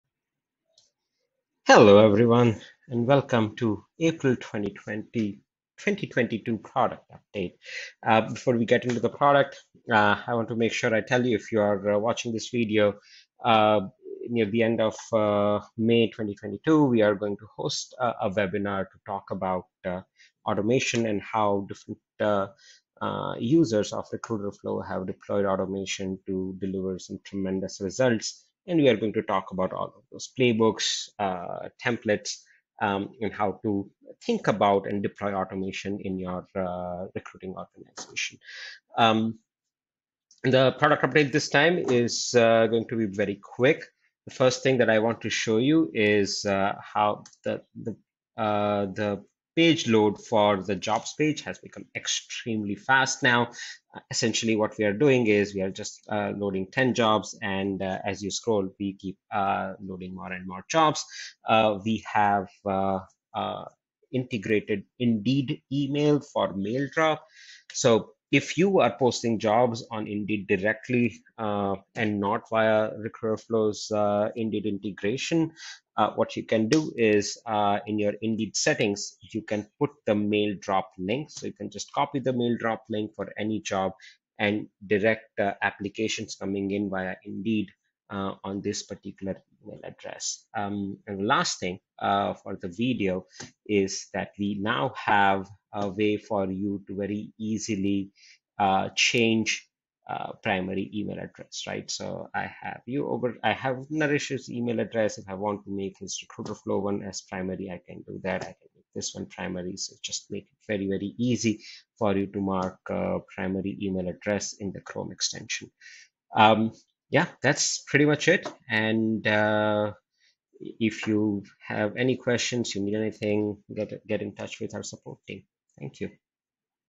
April 2022 - Product Update
Dive into our latest product update for April 2022!  Join us as we unveil exciting new features and enhancements designed to elevate your recruiting experience.

In this video, we'll showcase our commitment to speed and efficiency with blazing fast load times, ensuring you can navigate through Recruiterflow seamlessly.

Plus, we're thrilled to announce our integration with Indeed, now with maildrop functionality, streamlining your candidate sourcing process like never before.

Don't miss out on these game-changing enhancements. Watch the video now and stay ahead of the curve with Recruiterflow's latest innovations!